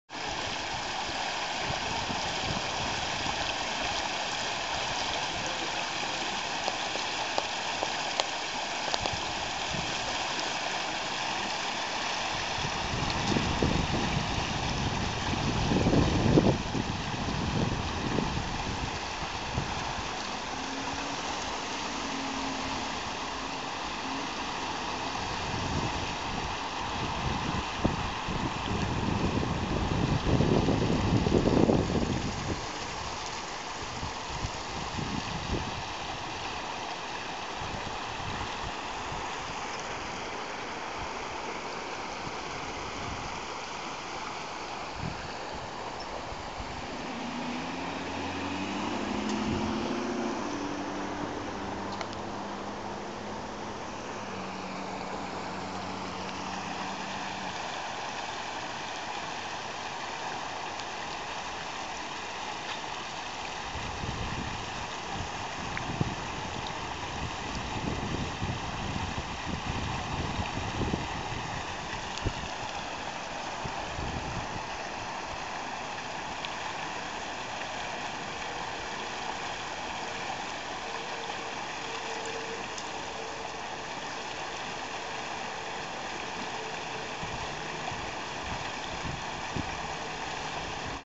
Sinkhole in Oakland
59th and Canning in Oakland, CA, a sinkhole erupted on the day after Christmas 2010.